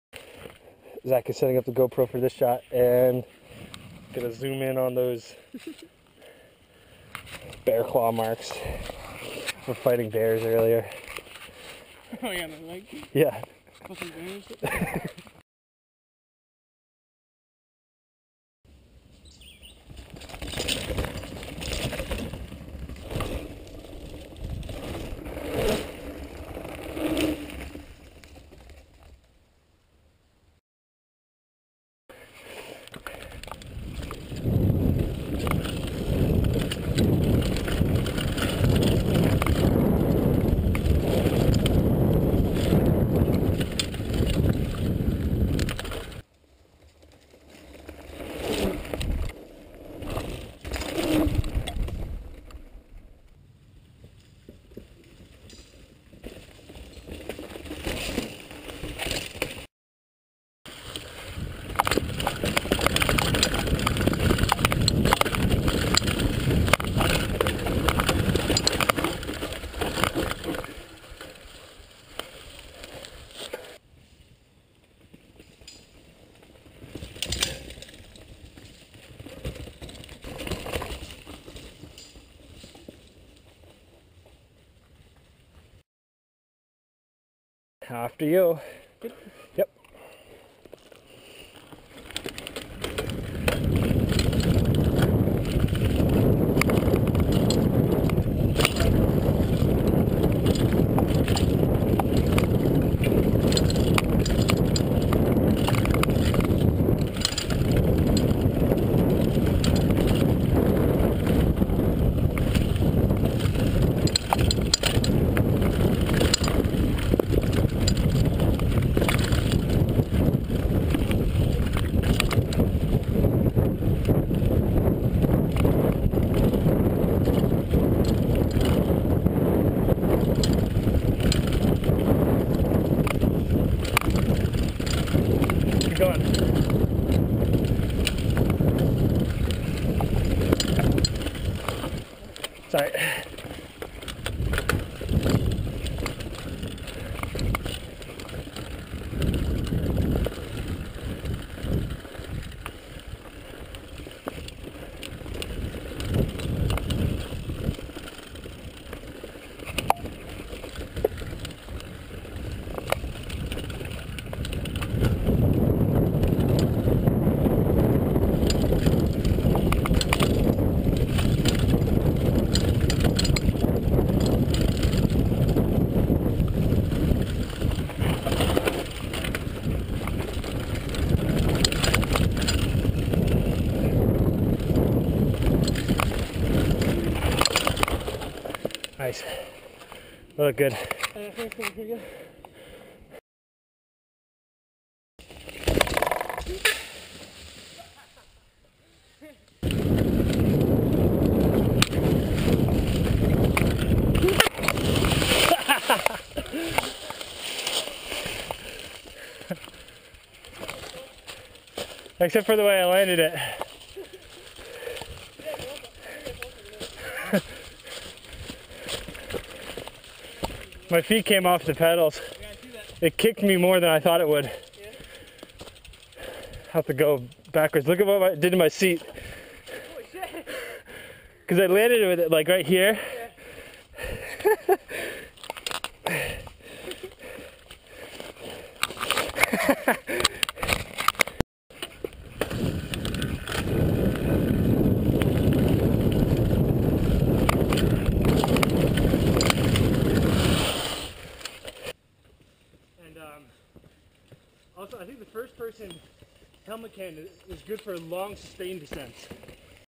GoPro HD Hero 960: Batchelor St. - Trail Riding
Granby, MA - Batchelor Street trails of Mt. Holyoke Range State Park.

Shot using 2 GoPro 960 cameras, edited in Windows Movie Maker (terrible).  Just goes to show, anyone can do this!